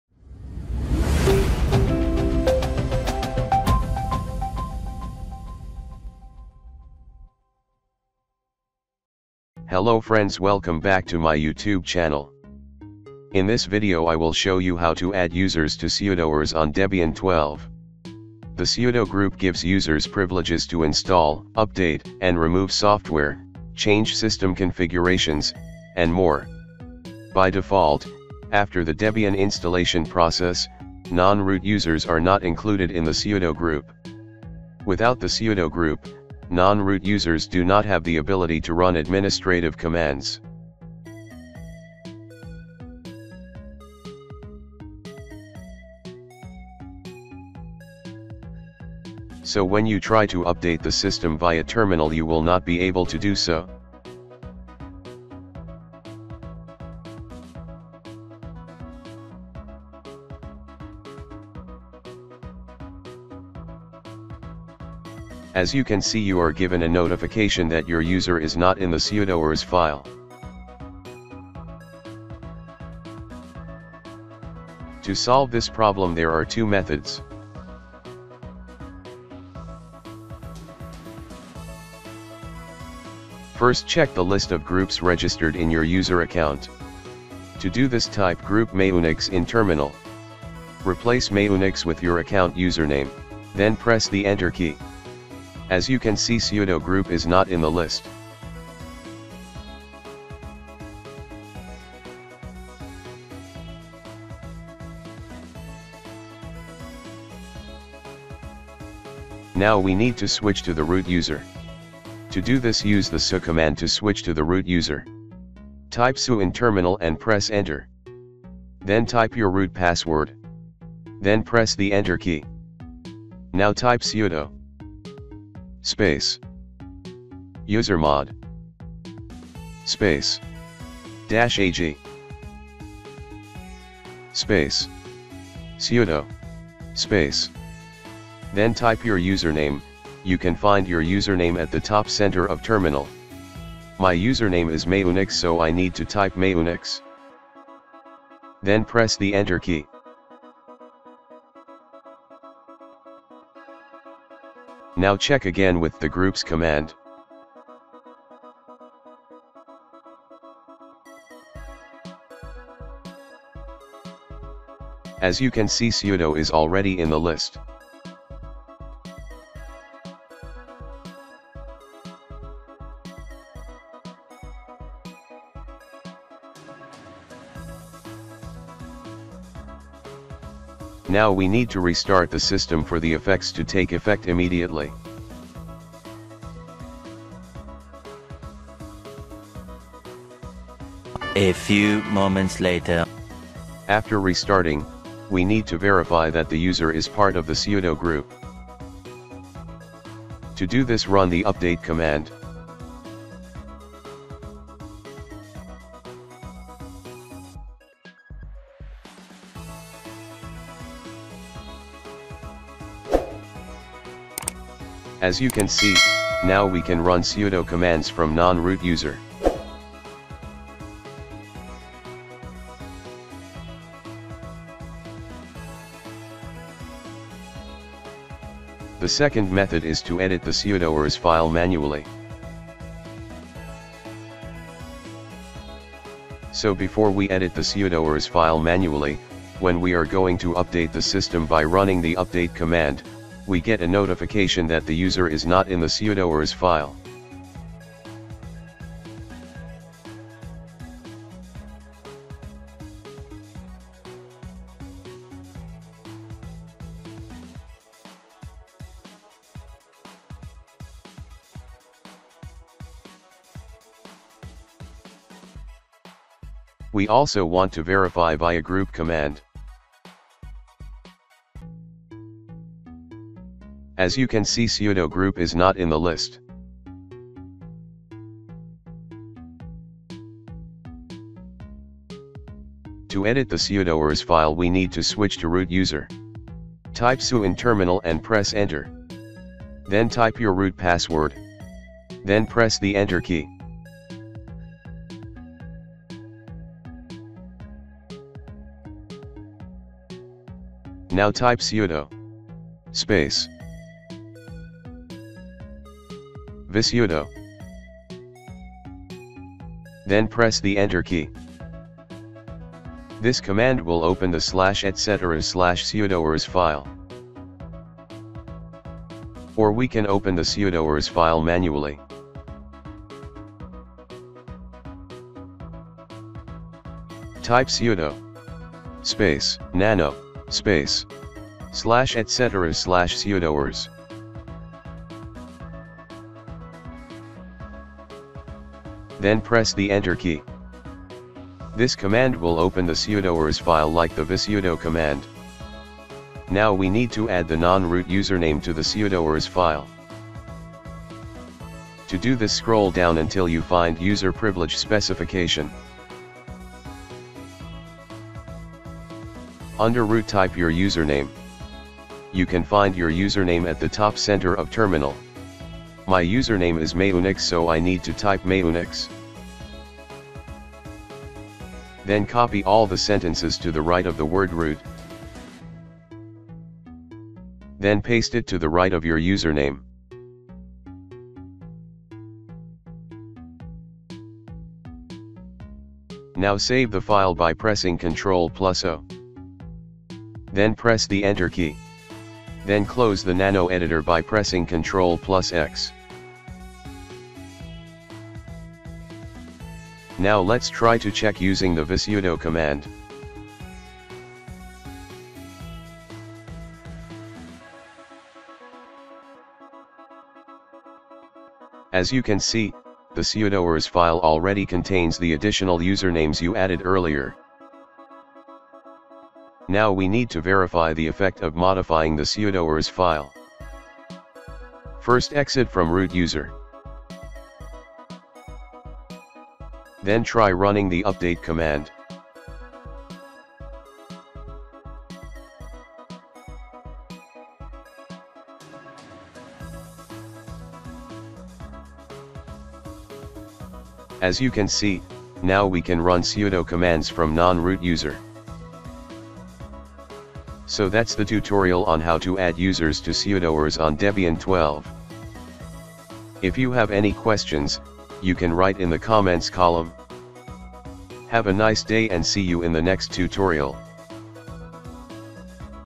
How to Add User to Sudoers in Debian 12
In this tutorial you can enable “sudo” on a user account through the following approach:

1. Using the usermod Command.
sudo usermod -aG sudo mayunix
(replace mayunix with your account username)

2. Via the sudoers File.
sudo visudo
or
sudo nano /etc/sudoers

Append “mayunix ALL=(ALL:ALL) ALL” to add a user in the sudoers file.